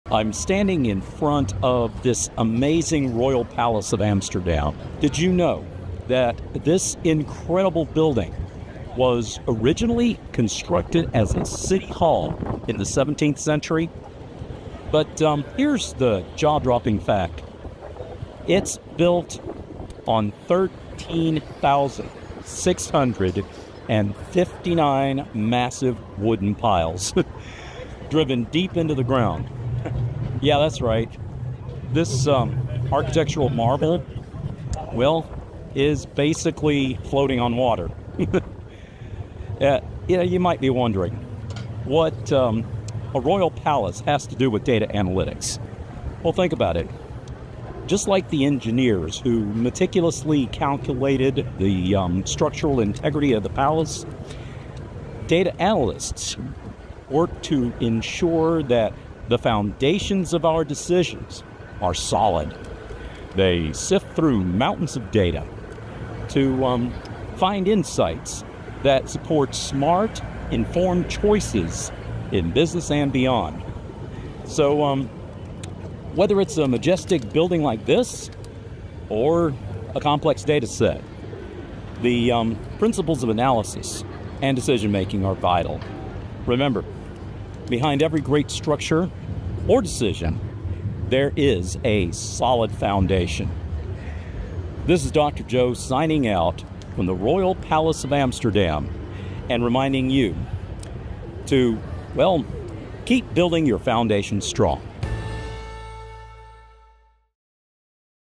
🏰Amsterdam Palace Data📊
17th century Amsterdammers built a palace on floating piles. What foundations do your data rest upon?

Follow us for more insights on InsightJam.com!